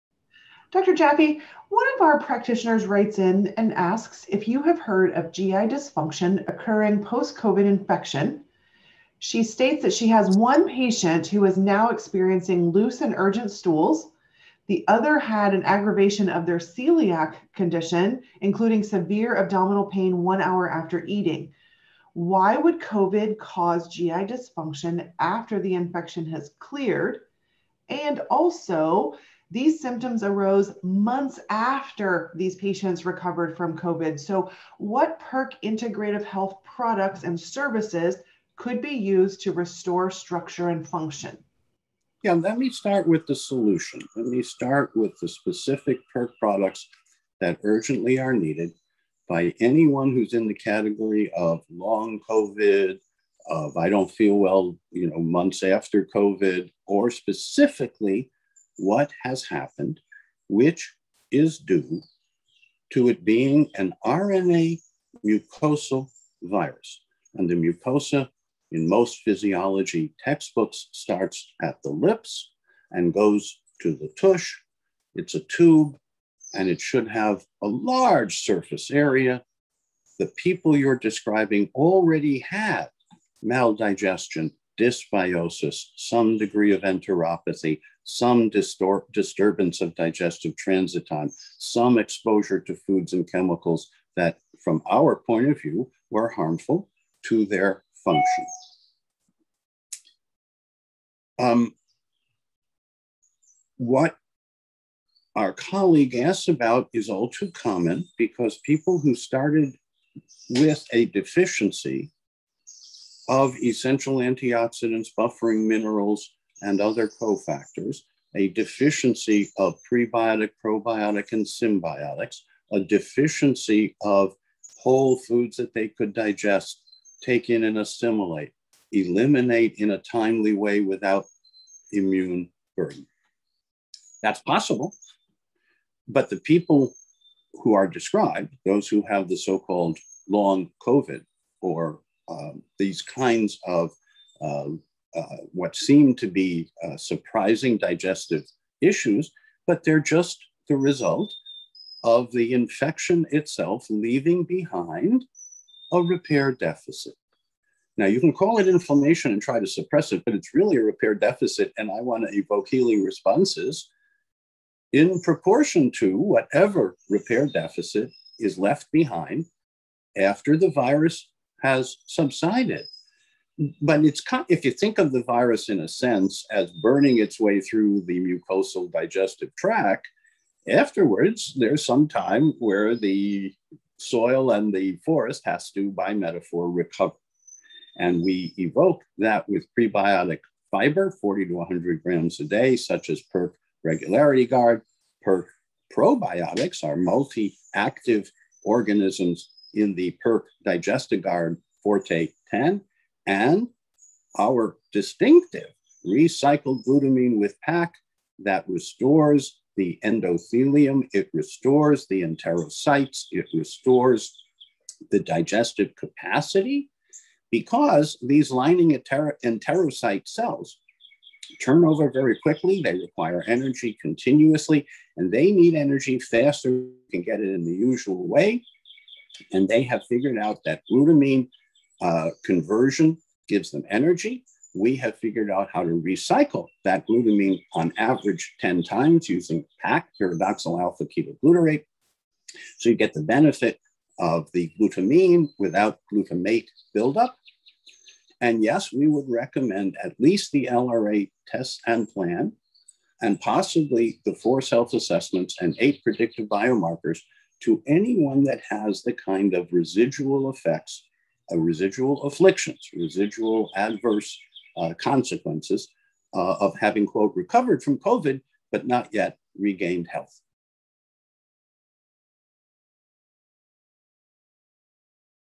GI Dysfunction Occurring Post COVID Infection
On this edition of "Ask Dr. J", a practitioner asks Dr. Jaffe if he has ever heard of GI dysfunction occuring post-COVID infection. The practitioner goes on to say that she has one patient who is now experiencing loose, urgent stools. Another patient had aggravation of their Celiac Disease including severe abdominal pain 1 hour after eating. Why would COVID cause GI dysfunction after the infection has cleared months after they recovered from COVID? Come hear what Dr. J. has to say!